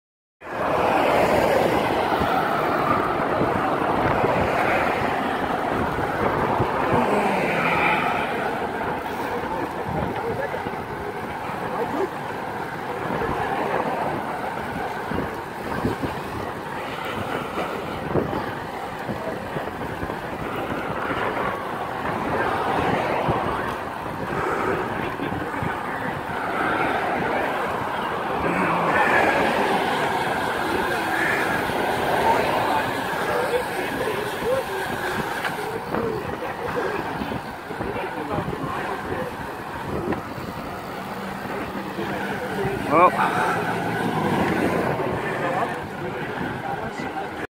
Philadelphia International Cycling Classic Pre Ride 2016
Very nice event this my second time doing it..love it